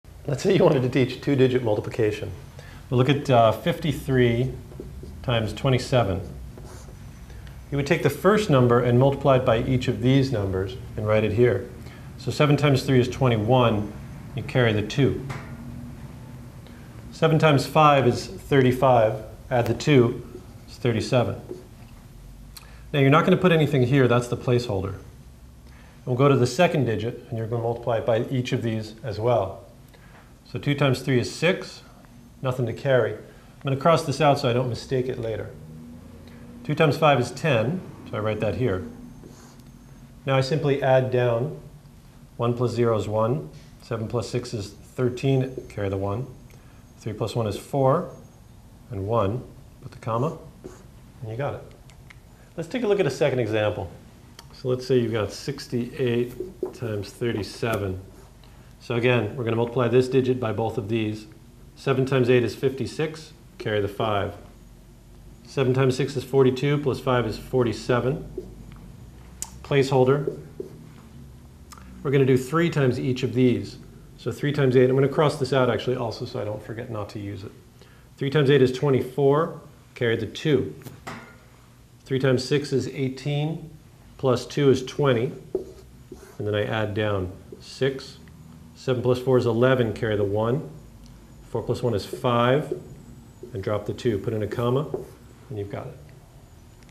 Math Fractions & Equations : How to Teach Two-Digit Multiplication
When multiplying two-digit numbers, take the first number and multiply it be each of the other numbers. Multiply two-digit numbers and learn about place holders with help from a math author and teacher in this free video on math lessons.